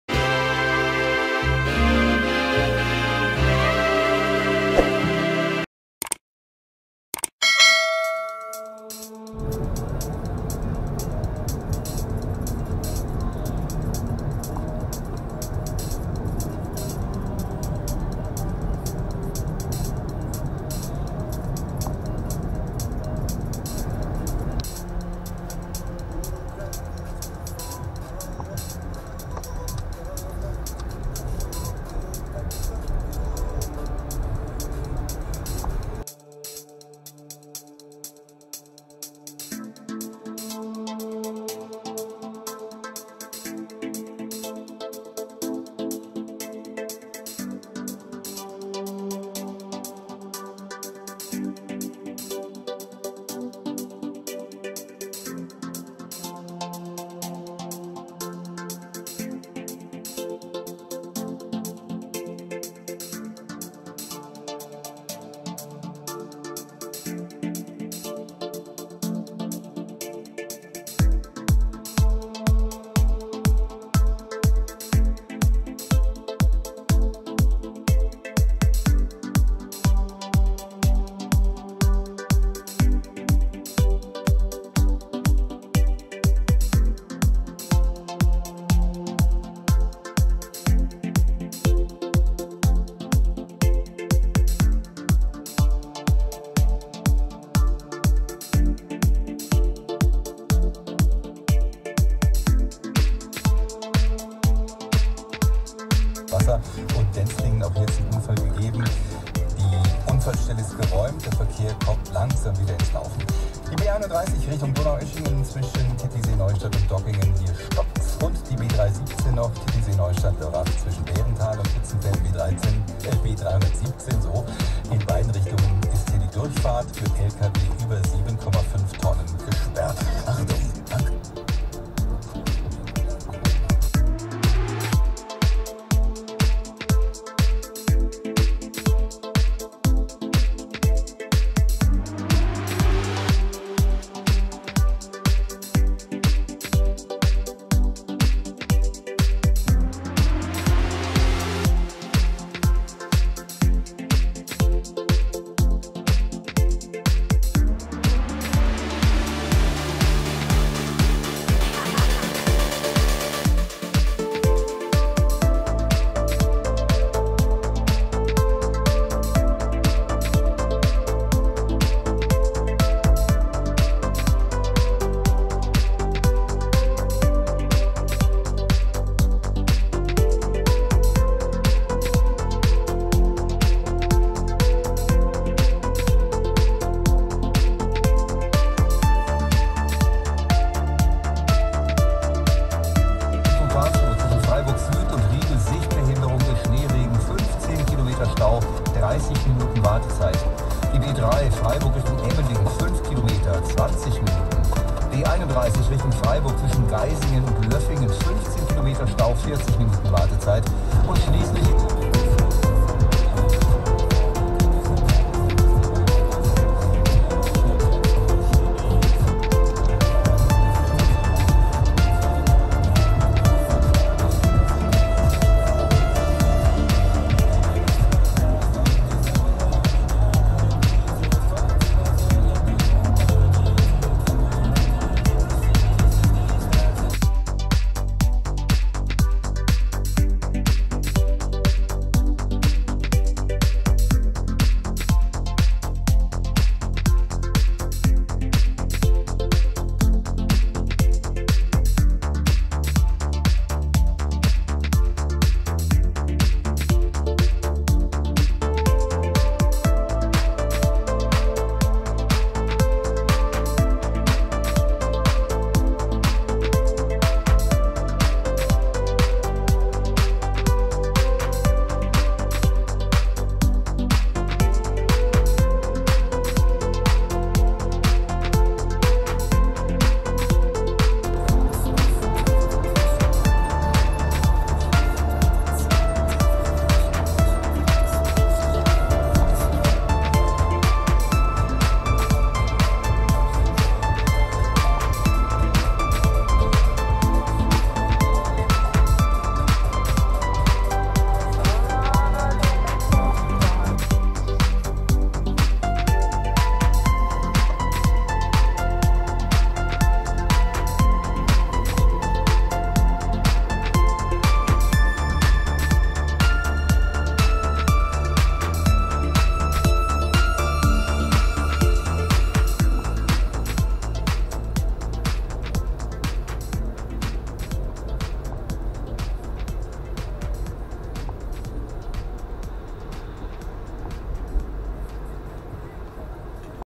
Winter ist da ! Поездка по первому снегу !
Musik von u_1banrwi1 auf Pixabay
Прокатился немножко по снежку, заодно испытал свою новую зимнюю резину из Юго-восточной Азии
Из минусов - расход топлива увеличился аж на 0,5 литра на 100
Из плюсов - цена (дешевле только даром :) , по накатанному мокрому снегу идёт как по рельсам (пешком ходить сложнее, ноги разъезжаются :)